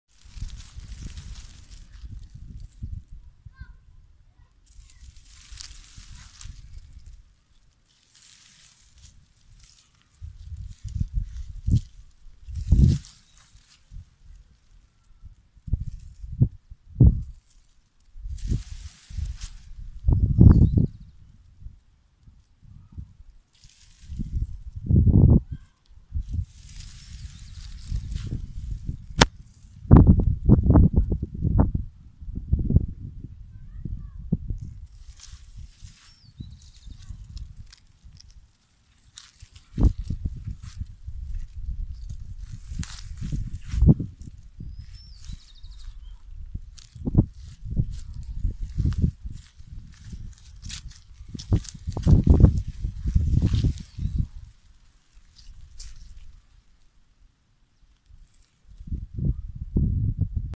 Impresionante manga de langosta invade el centro de Villa Unión
Una lectora del Periódico Sur Santiagueño envió imágenes de una gran manga de langosta voladoras en el centro de Villa Union Dpto Mitre.